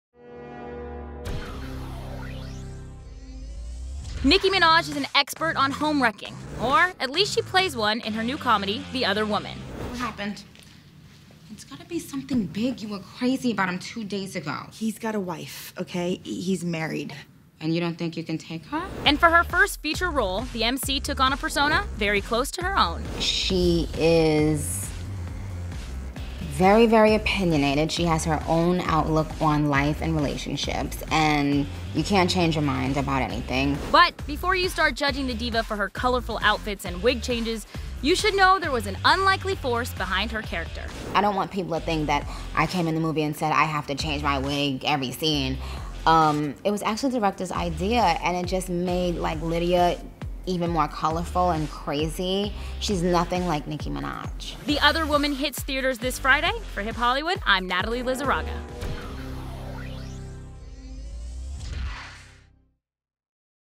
Nicki Minaj Ditches Her Natural Look For Movie Role - HipHollywood.com
Nicki Minaj opens up about her first feature role, playing a home wrecker, and her ever so changing hair styles.